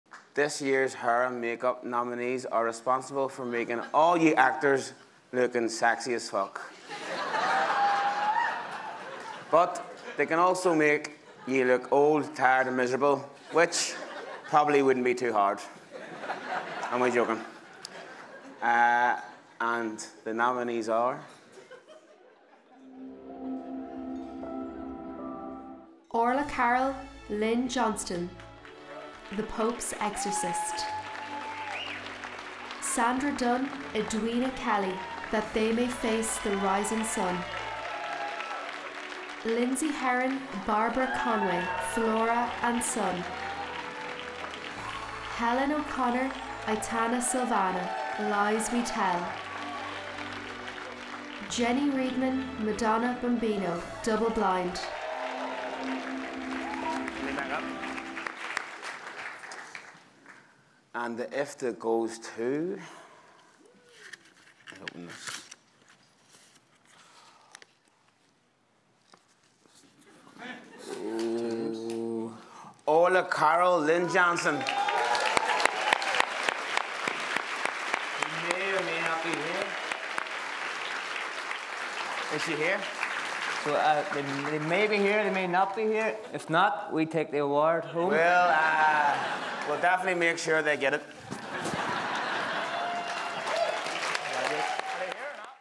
Orla Carroll, Lynn Johnston (The Pope's Exorcist) win Hair & Makeup - Presented by Kneecap
Kneecap present the IFTA Award for Hair & Makeup to Orla Carroll and Lynn Johnston for their work on The Pope's Exorcist, at the 21st Anniversary IFTA Awards on 20th April 2024.